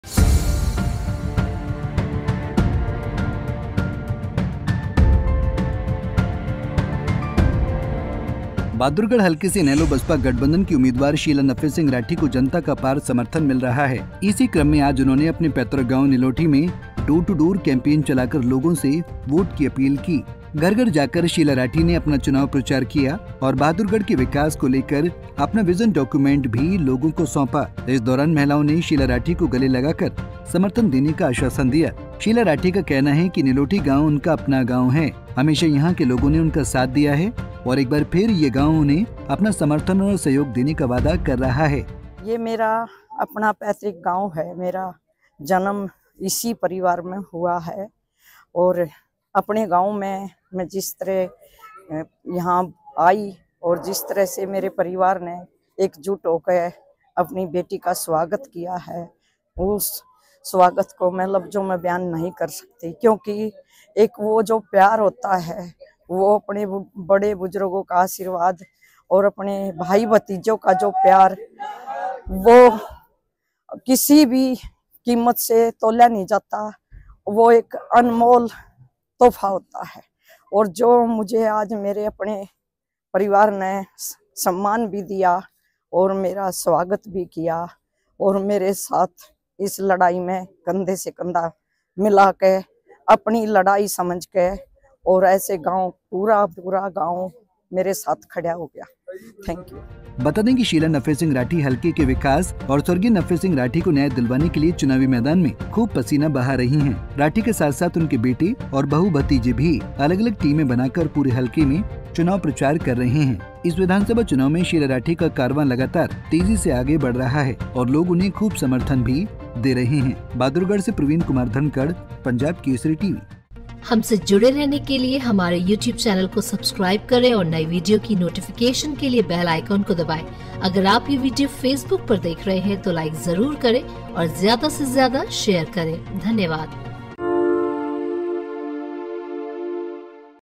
निलोठी गांव में Sheela Rathi ने चलाया डोर टू डोर कैंपेन, स्वागत में लोगों का हुजूम
बहादुरगढ़ हलके से इनेलो बसपा गठबंधन की उम्मीदवार शीला नफे सिंह राठी को जनता का अपार समर्थन मिल रहा है. इसी क्रम में आज उन्होंने अपने पैतृक गांव निलोठी में डोर टू डोर कैंपेन चलाकर लोगों से वोट की अपील की...